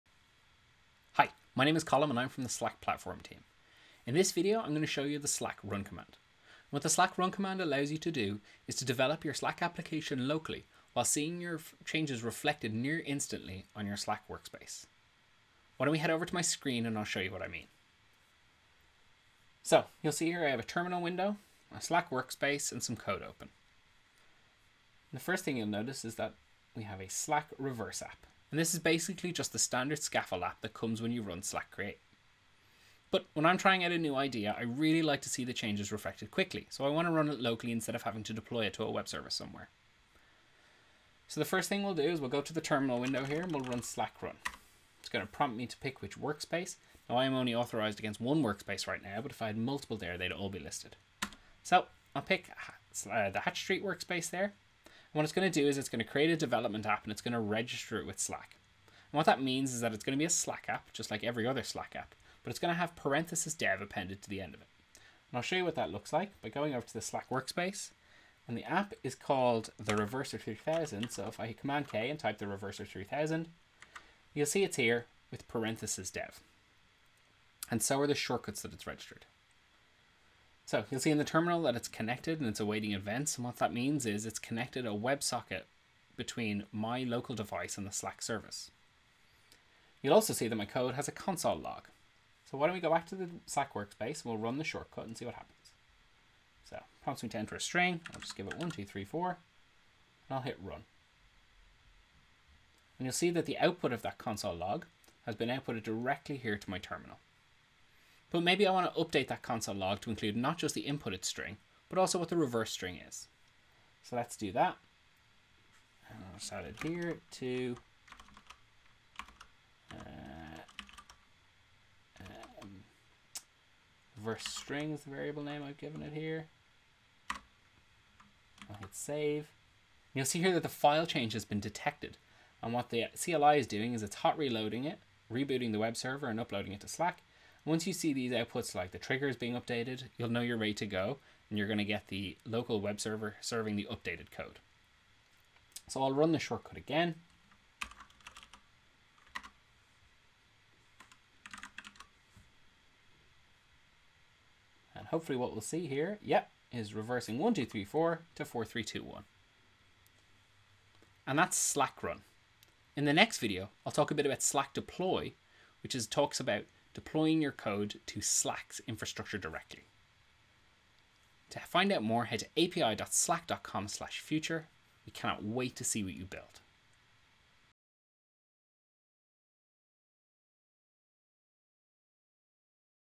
Developing with hot reloads using slack run | Slack Platform
When you’re working on a new idea for a slack app, fast iterations are a really useful tool on your toolbelt. With the hot reloading built into the Slack CLI, you can update your development app without redeploying once.

00:00 Introduction
00:40 Registering a development app
01:30 Viewing your application’s logs in your terminal
01:47 Hot reloading your development app